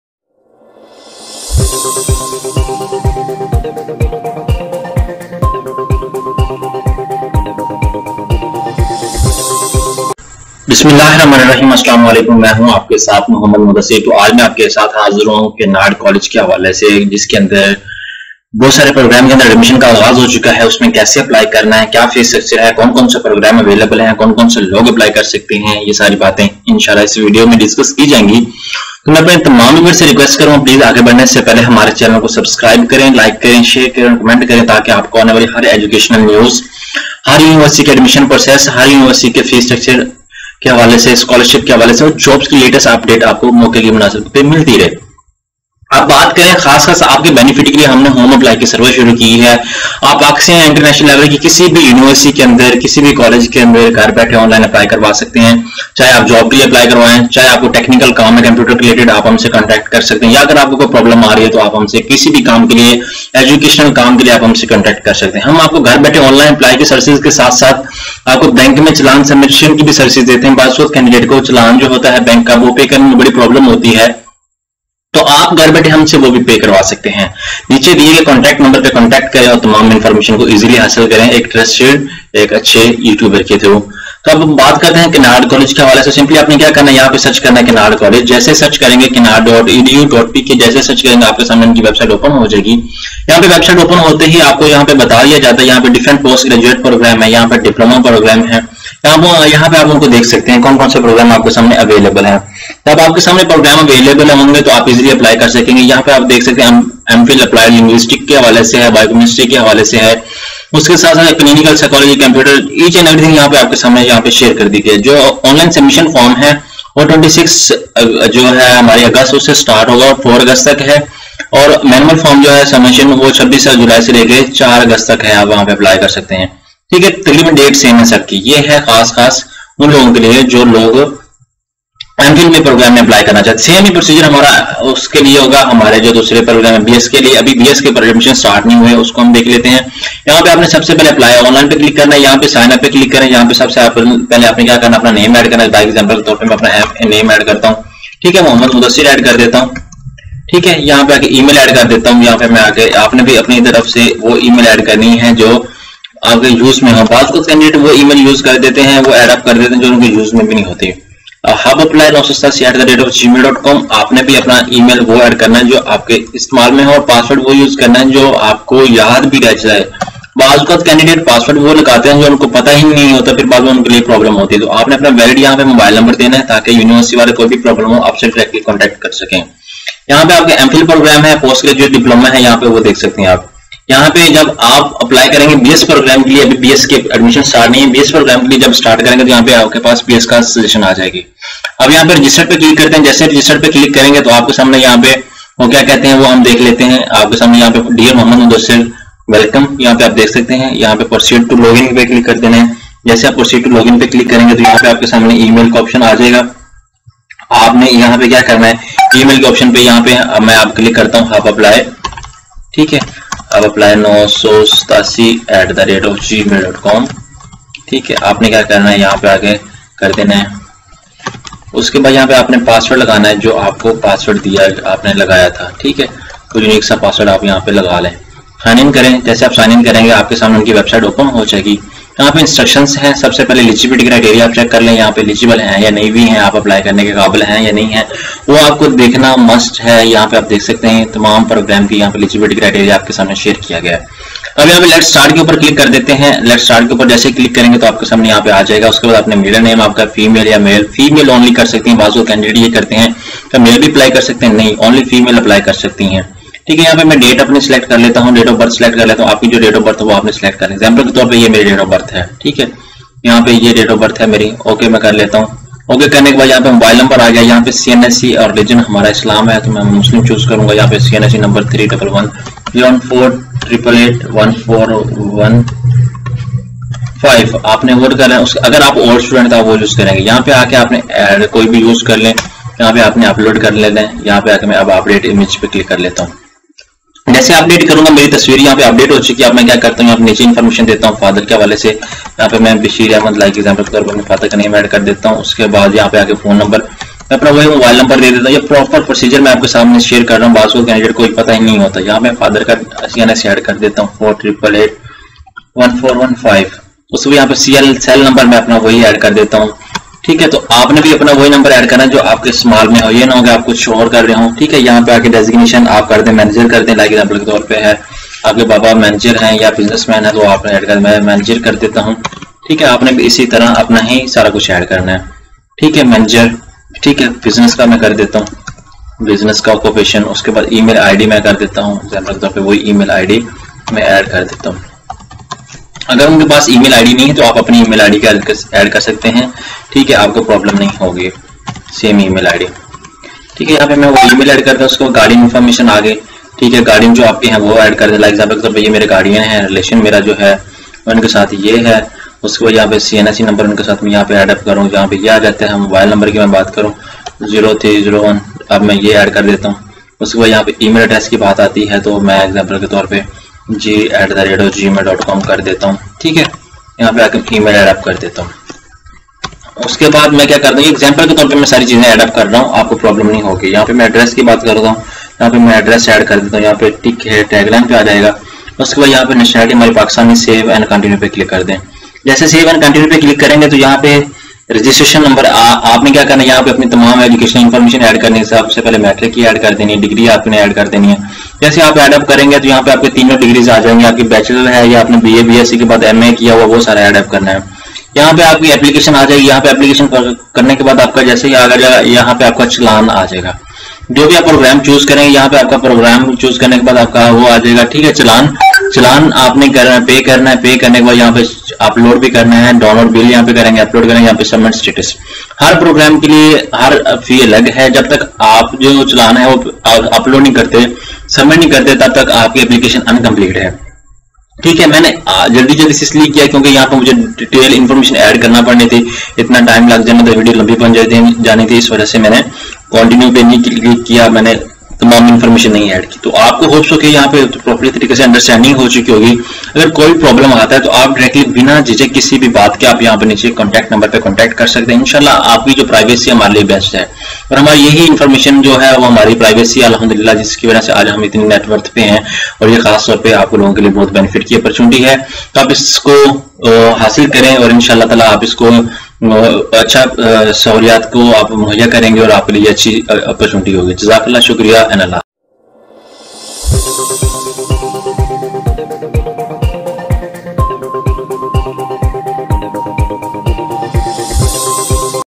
How to apply in Kinnard College for Women fall 2021 | online Apply | fee structure | full detail
رجسٹریشن
چالان جمع کروانا
 پوسٹ کروانا
ڈیٹا انٹری